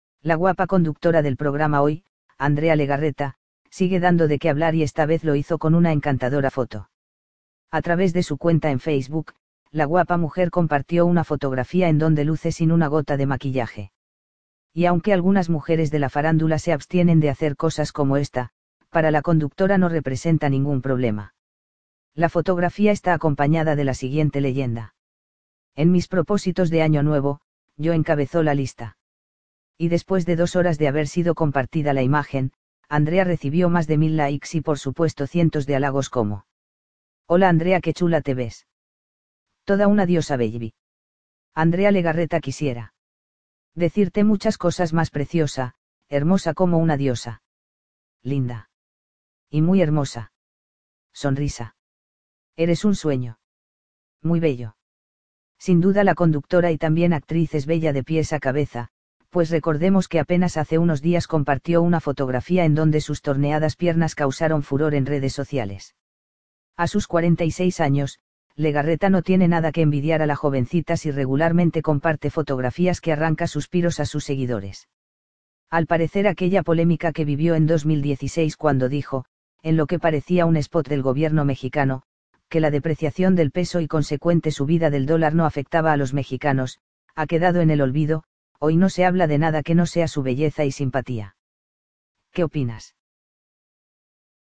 Andrea Legarreta posa sin maquillaje y sorprende las redes!
La guapa conductora del programa ‘Hoy’, Andrea Legarreta, sigue dando de qué hablar y está vez lo hizo con una encantadora foto.

A través de su cuenta en Facebook, la guapa mujer compartió una fotografía en donde luce sin una gota de maquillaje. Y aunque algunas mujeres de la farándula se abstienen de hacer cosas como esta, para la conductora no representa ningún problema.

La fotografía está acompañada de la siguiente leyenda:

En mis propósitos de año nuevo, yo encabezó la lista’

Y después de dos horas de haber sido compartida la imagen, Andrea recibió más de mil ‘likes’ y por supuesto cientos de halagos como:

Hola Andrea que chula te ves’

Toda una diosa beybi’

Andrea Legarreta quisiera ……. .. Decirte muchas cosas más preciosa, hermosa como una diosa’

Linda. Y muy hermosa. Sonrisa. Eres un sueño. Muy bello’

Sin duda la conductora y también actriz es bella de pies a cabeza, pues recordemos que apenas hace unos días compartió una fotografía en donde sus torneadas piernas causaron furor en redes sociales.

A sus 46 años, Legarreta no tiene nada que envidiar a la jovencitas y regularmente comparte fotografías que arranca suspiros a sus seguidores.

Al parecer aquella polémica que vivió en 2016 cuando dijo (en lo que parecía un spot del gobierno mexicano) que la depreciación del peso y consecuente subida del dólar no afectaba a los mexicanos, ha quedado en el olvido, hoy no se habla de nada que no sea su belleza y simpatía. ¿Qué opinas?